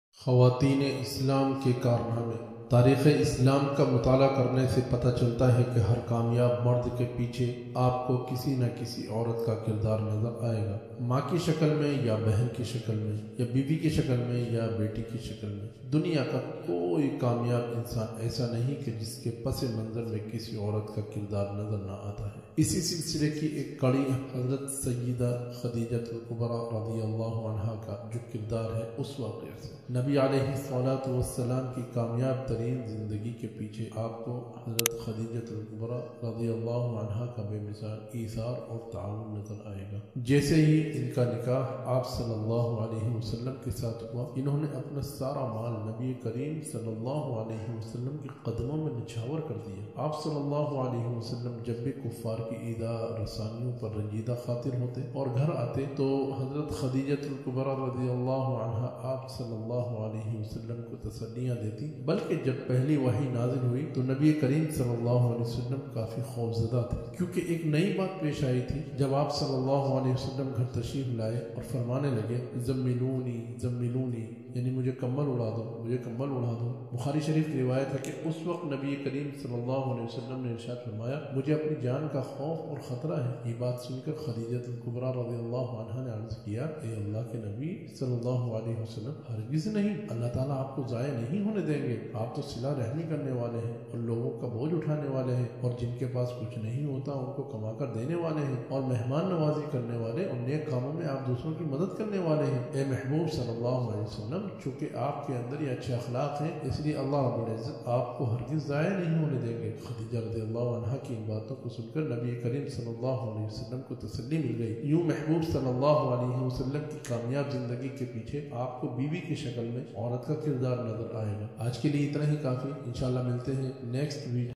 Hazrat Khadeeja R.A || خواتین اسلام کے کارنامے || Qari Siddeeque Sahab
اسلام نے عورت کو بہت اونچا مقام عطا کیا ہے۔
یہ کہا جاتا ہے کہ ہر کامیاب مرد کے پیچھے ایک عورت کا ہاتھ ہوتا ہے۔
Qari Siddeeque Sahab Qasmi